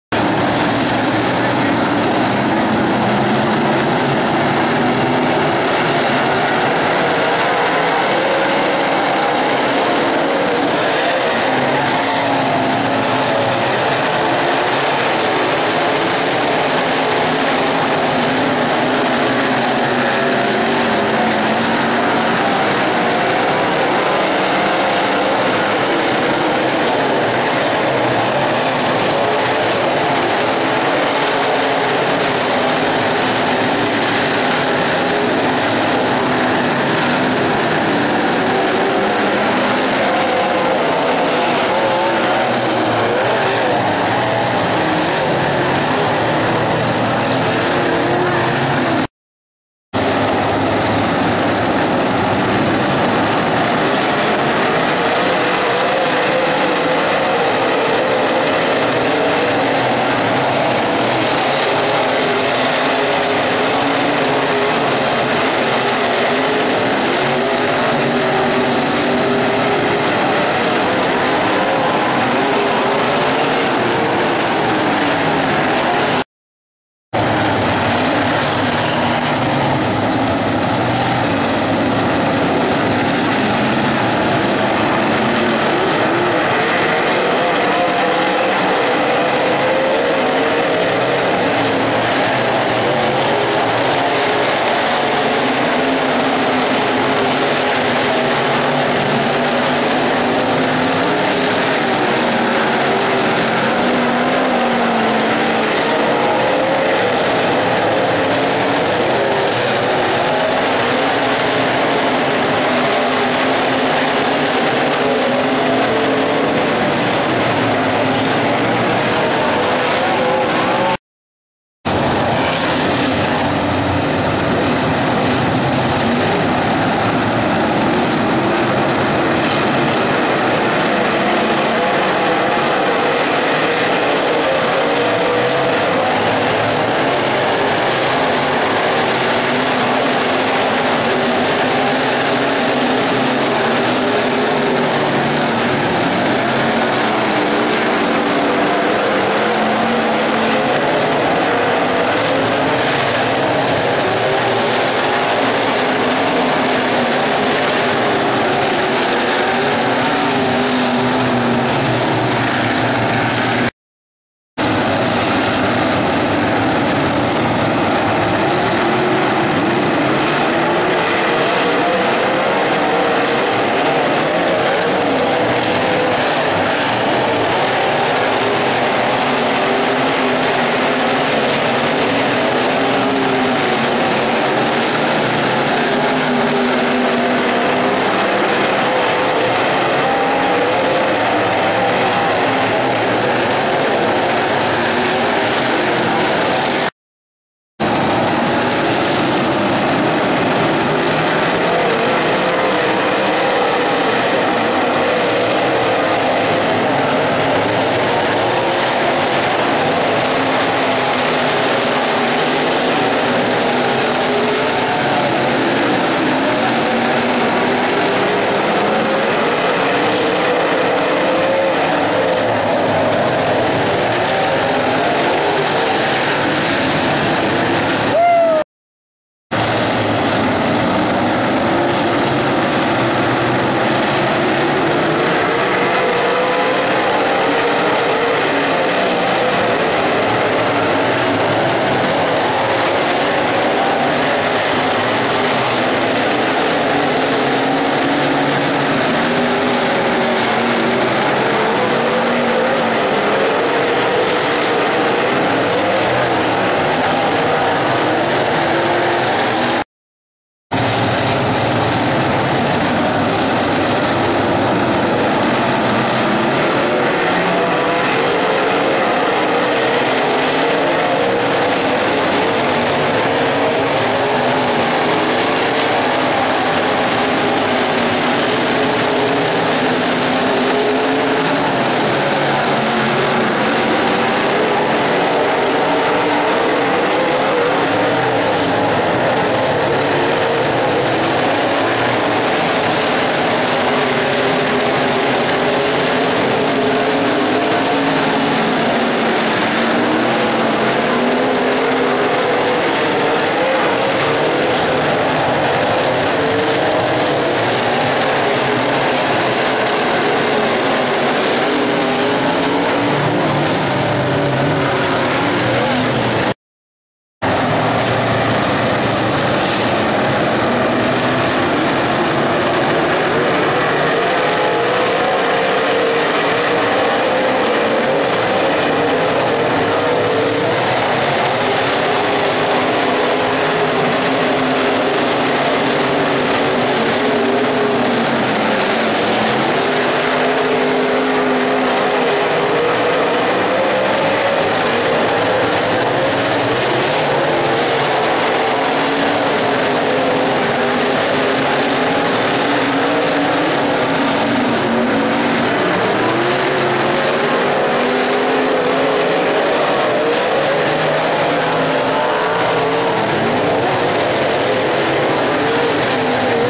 Modified Feature From 201 Speedway,10/6/2012.
Modified feature from 201 Speedway,10/6/2012. Feature was cut short due to time limit.